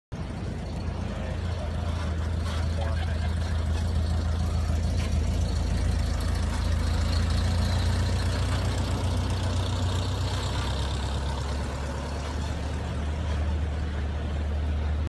VW Kombi in Main Beach carpark, Byron Bay
@Byronkombilimo a 1964 Volkswagen 11 window bus in classic blue & white with safari split windscreens, cruising through the carpark on a surfcheck tour of Byron Bay.